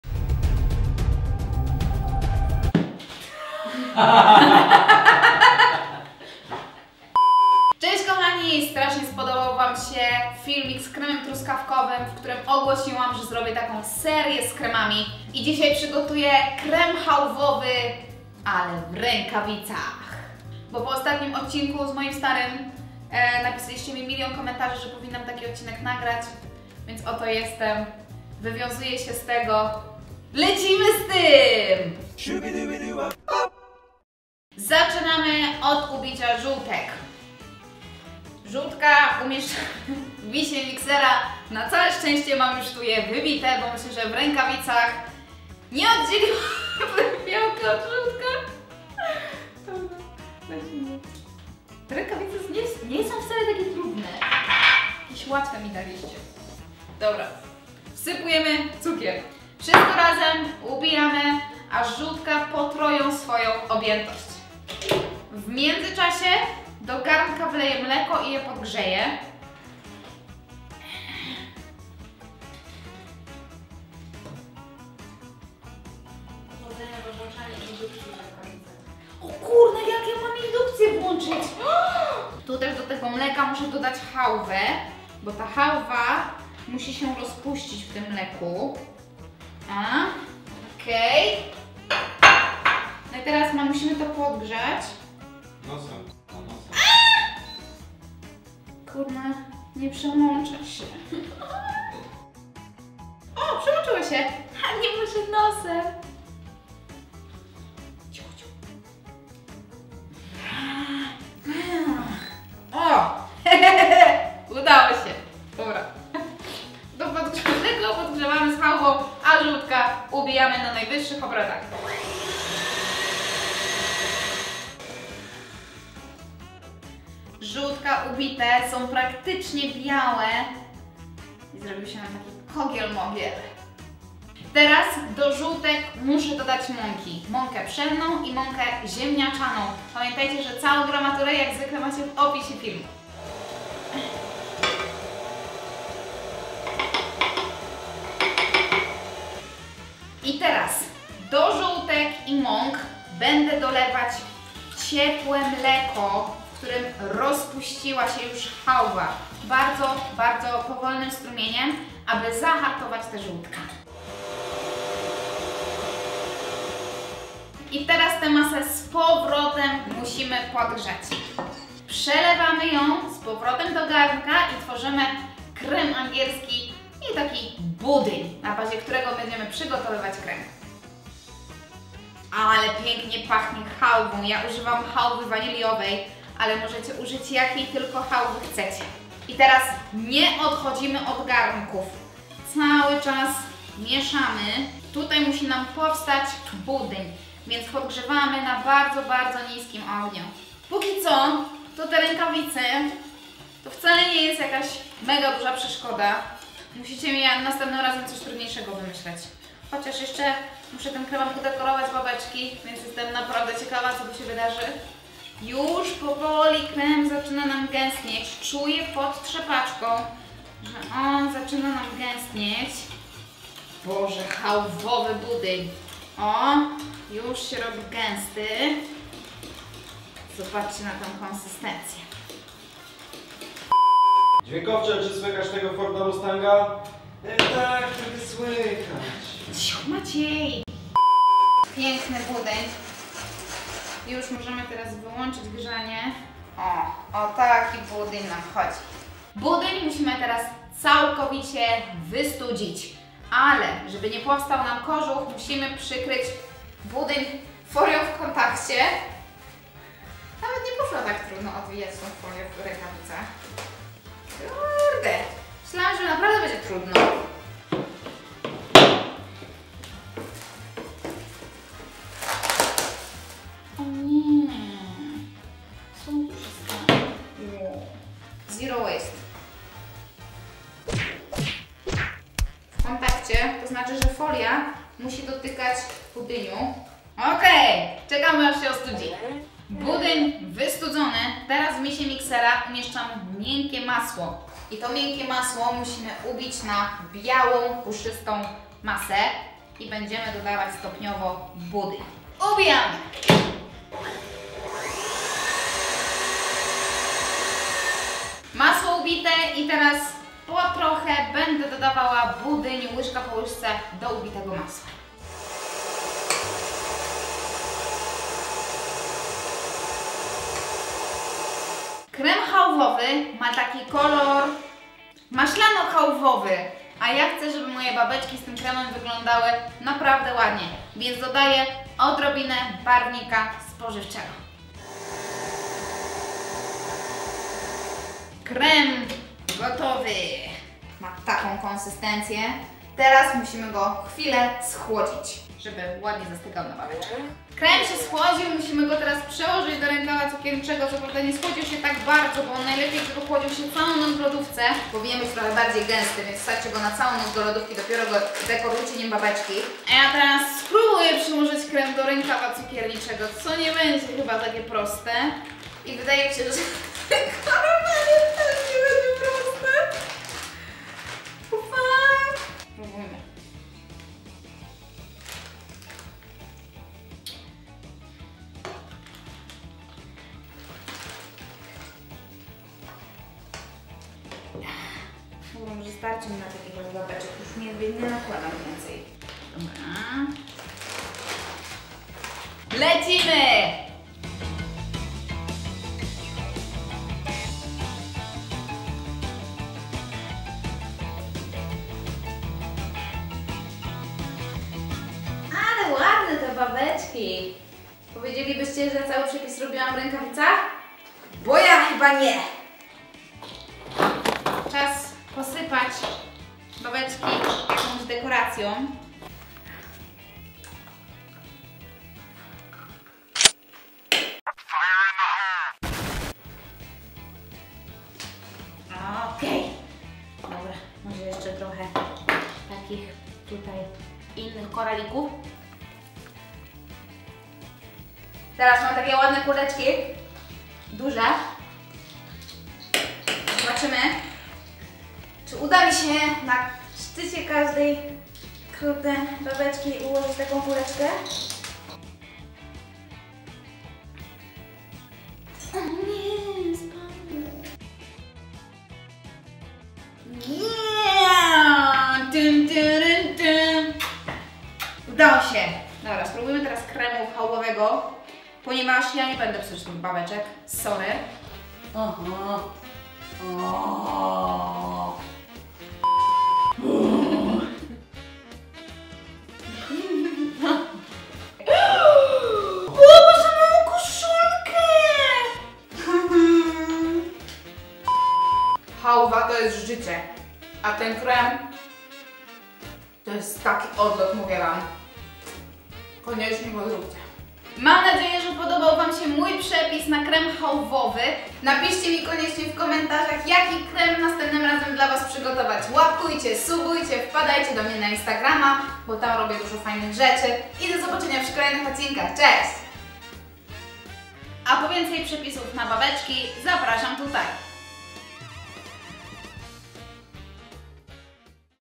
CAŁY PRZEPIS W RĘKAWICACH! Robię KREM chałwowy!
Cześć! Dzisiaj mam dla Was odcinek z wyzwaniem, o które dużo osób prosiło. Cały przepis robię w rękawicach! A zdecydowałam się zrobić krem chałwowy, który jest idealny do tortu lub babeczek. Jeśli nie wiecie, jak go zrobić to zapraszam do oglądania no i sprawdźcie przy okazji jak mi poszło :)

Składniki:
500ml mleka
3 żółtka
40g cukru
40g maki pszennej
40g maki ziemniaczanej
200g miękkiego masła
300g chałwy